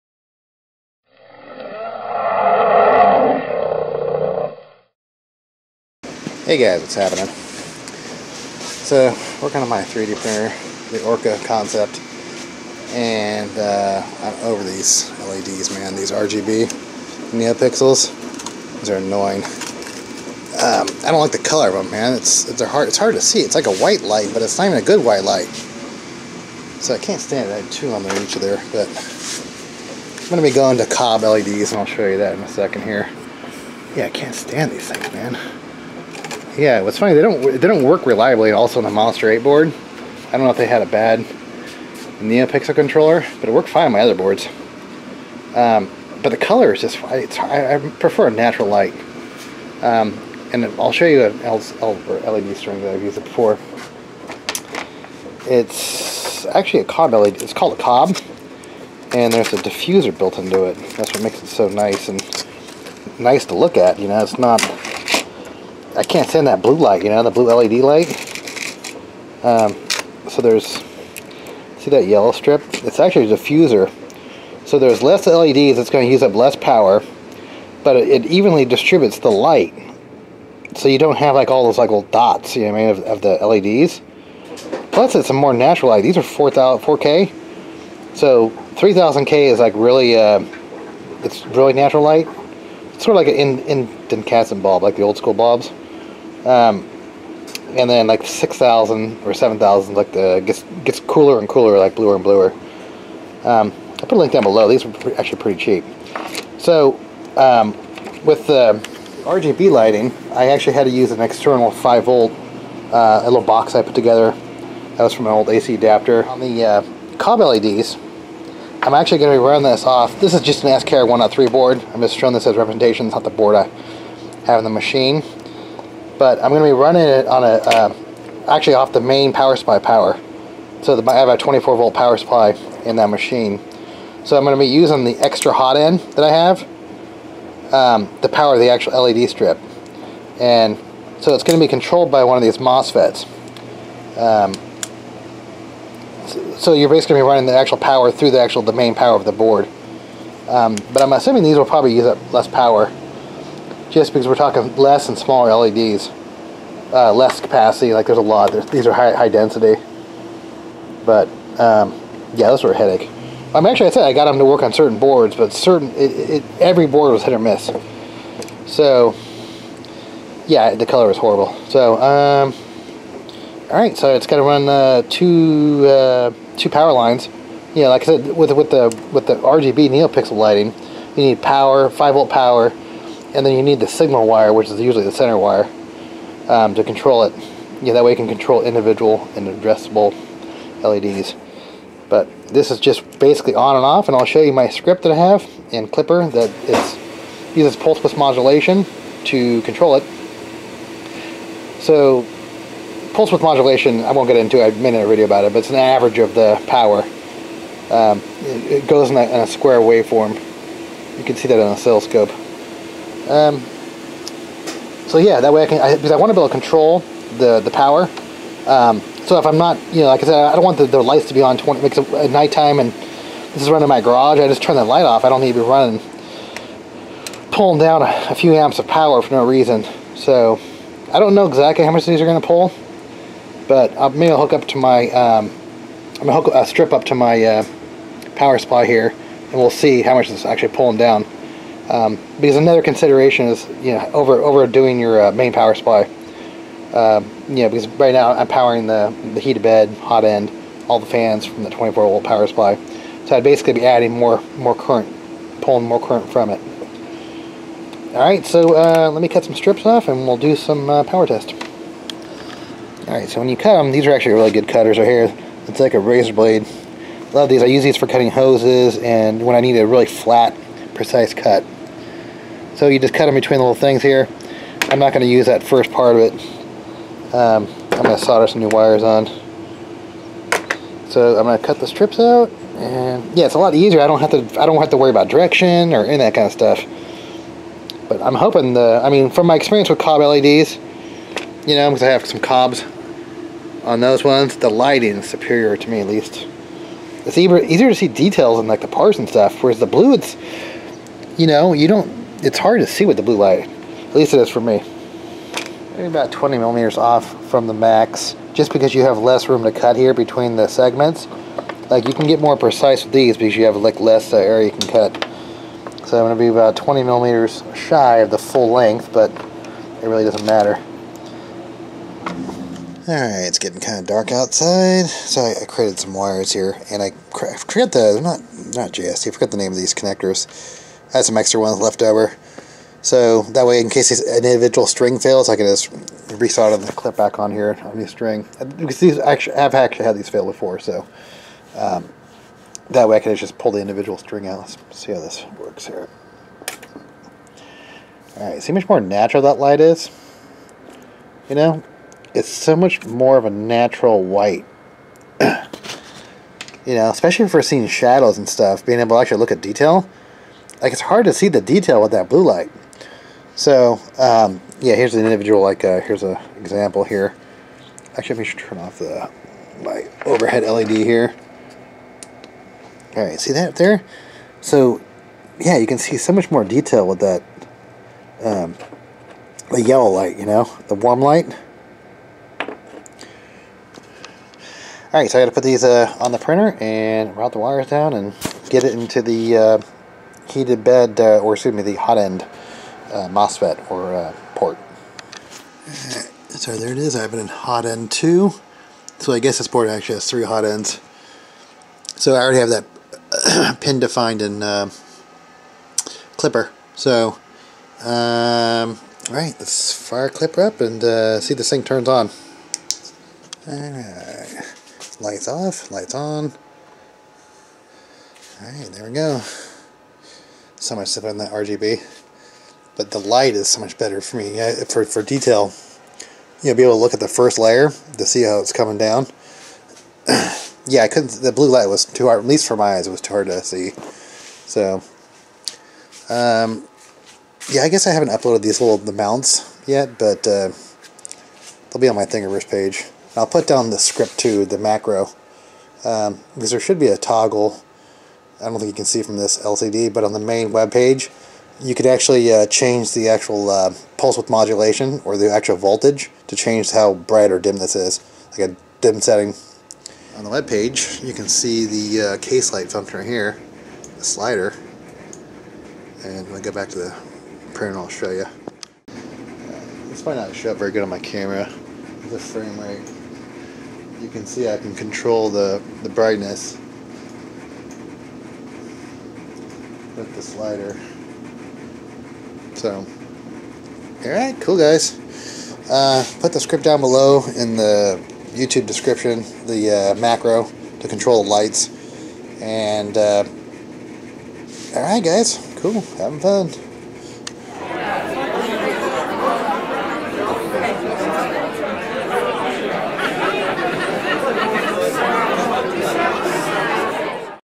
Klipper COB LED case light control macro running on extra hot end port mosfet
I'm over Neopixel RGB lighting. The white light too blue for my taste. I prefer the natural light of a COB LED. The diffuser evenly distributes the light. The roll was only $6.99 on amazon and allot easier to set them Neopixel. The script has an on and off button. Also a slider to control brightness.

Strip On Amazon

Cutters on Amazon

Macro for LED control. You must match output pin of your board

# Chamber Lighting
[output_pin caselight]
pin: PA3
max_power: 1.0
kick_start_time: 0.5
pwm: true
shutdown_value: 0
value: 100
cycle_time: 0.01
scale: 100

# Lighting Control
[gcode_macro lights_off]
gcode:
    SET_PIN PIN=caselight VALUE=0

[gcode_macro lights_on]
gcode:
    SET_PIN PIN=caselight VALUE=100

[gcode_macro lights_toggle]
gcode:
    {% if printer["output_pin caselight"].value == 100 %}
        lights_off
    {% else %}
        lights_on
    {% endif %}

If you found this video helpful or would like help fund future experiments.